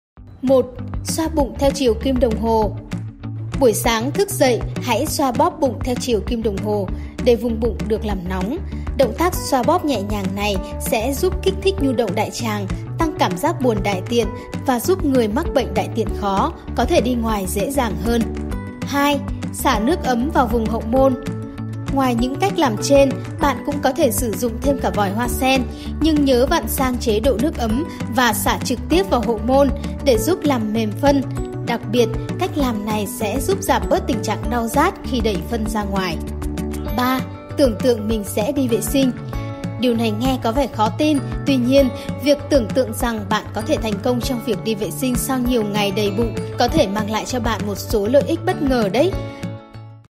ĐỐC TƠ TRĨ: MẸO ĐI VỆ SINH SAU 30 GIÂY VỚI MẸO ĐƠN GIẢN, TRẺ CON CŨNG LÀM ĐƯỢC
Không cần phải đỏ mặt tía tai, khi bị táo bón hãy đưa ngón tay lên rồi ấn vào một điểm này giúp bạn thổi bay ngay trong vài phút
Bất kì ai trong chúng ta đã từng bị táo bón đều hiểu sự bất tiện mà nó đem lại. Việc phải ngồi hàng giờ trong nhà vệ sinh với các triệu chứng đau rát hẳn sẽ khiến bạn mệt mỏi vô cùng. Bên cạnh việc bổ sung chất xơ từ rau củ quả, uống nước nhiều, tập thể dục, có một cách để trị táo bón khá đơn giản mà không tốn kém gì, đó chính là bấm huyệt Thương Dương. Chỉ vài phút thôi, việc đi đại tiện với bạn sẽ thật dễ dàng. Cùng theo dõi hướng dẫn của chuyên gia ngay sau đây bạn nhé!